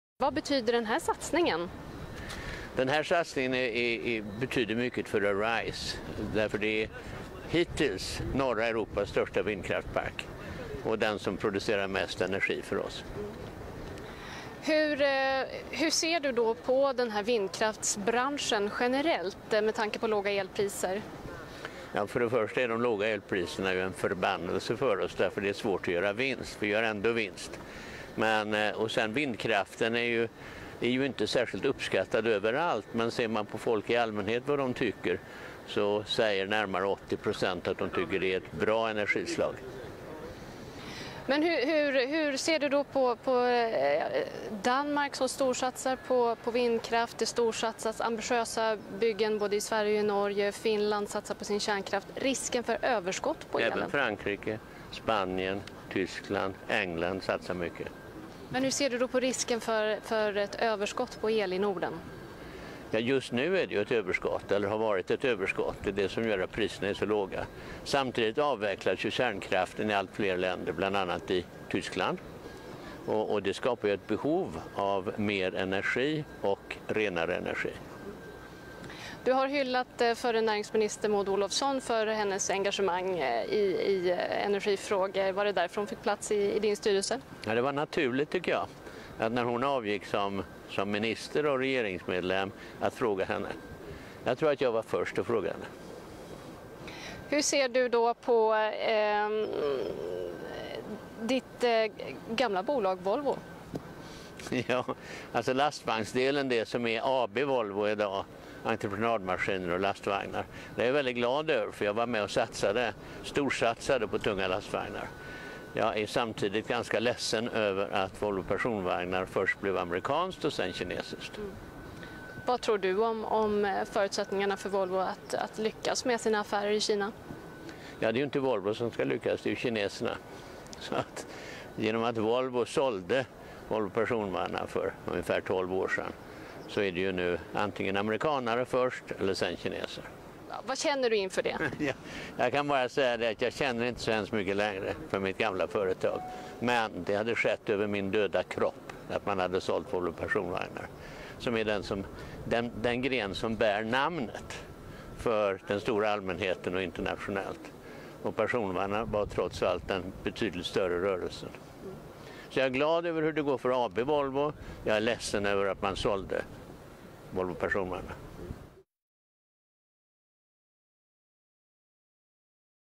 PG Gyllenhammar
Under tisdagen invigde näringslivsprofilen PG Gyllenhammar, Nordens största landbaserade vindkraftverk. Noterade Arise Windpower har byggt 66 vindkraftverk i Jädraås, i Ockelbo kommun som årligen ska producera motsvarande 114 lägenheters elförbrukning per år.